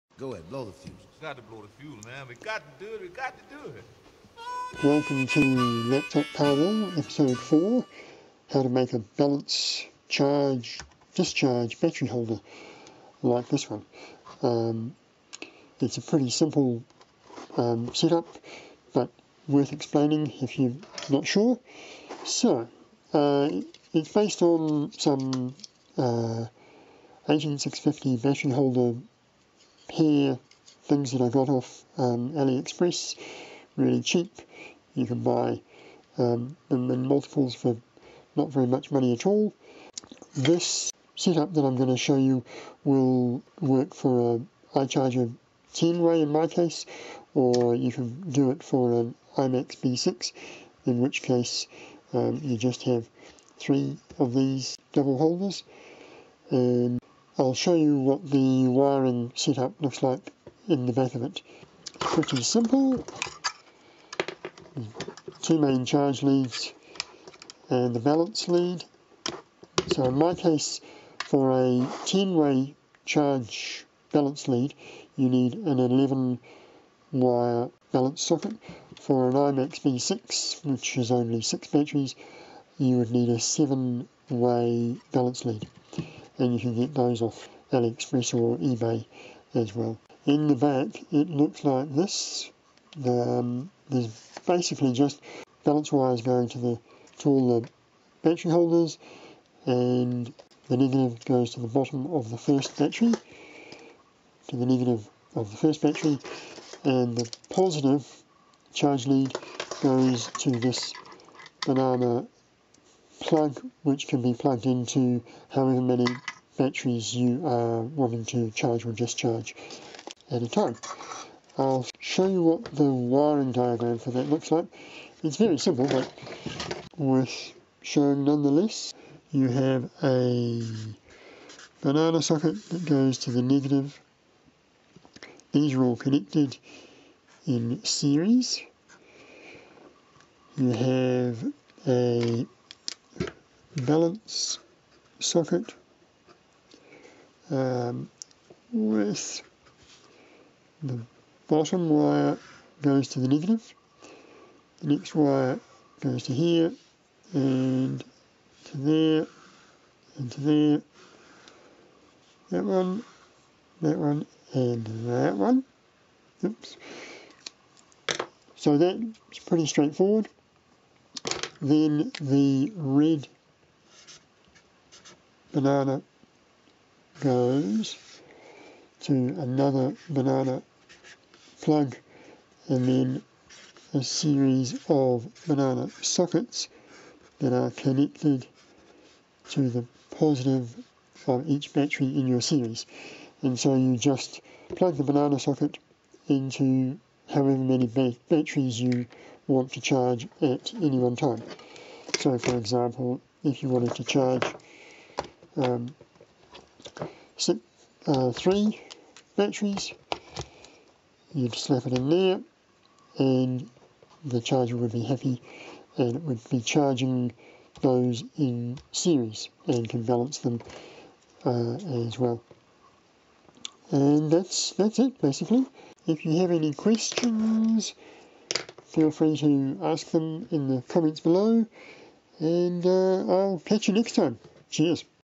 Laptop Powerwall 04 - 18650 10-cell charger
In this episode I show how to build a balance charger for multiple 18650 batteries.